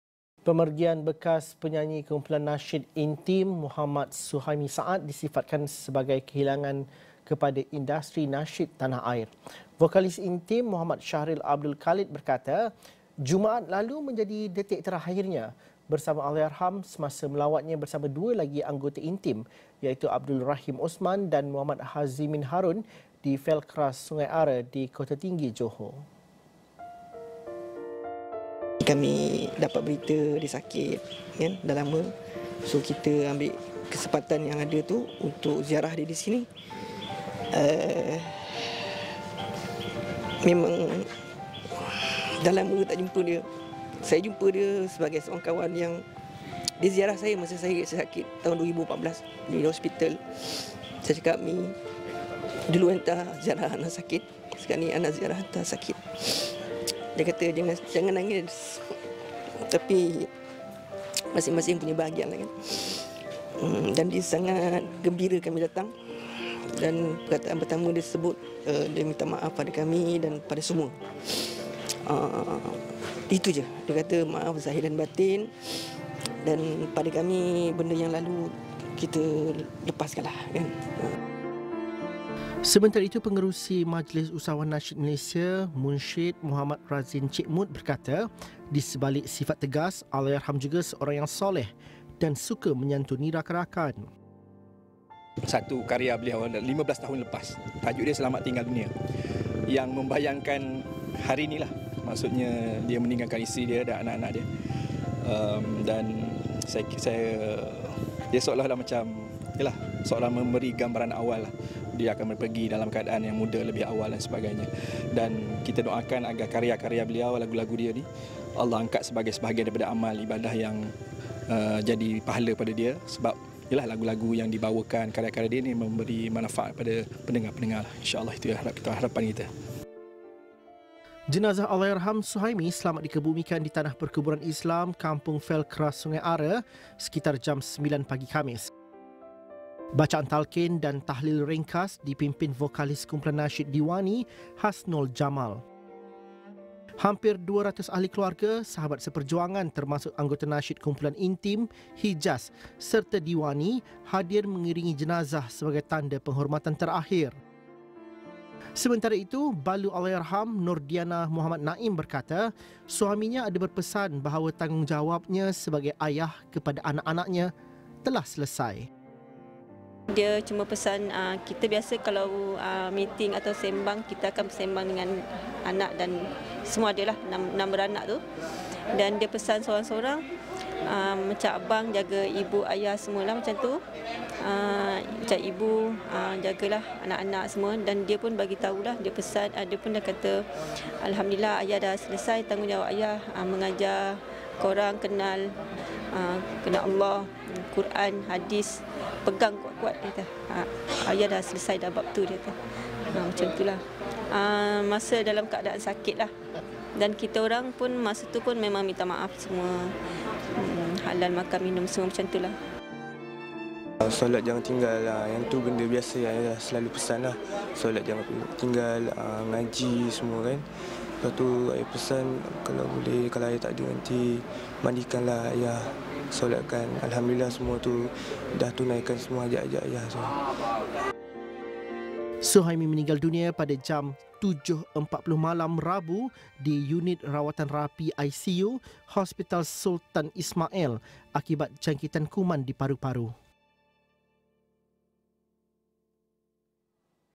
Pemergian Suhaimi kehilangan besar kepada industri nasyid
Jenazah Mohd Suhaimi Saad selamat dikebumikan sekitar jam 9 pagi tadi di Felcra Sungai Ara, Kota Tinggi Johor. Kira-kira 200 hadir pengebumian Allahyarham.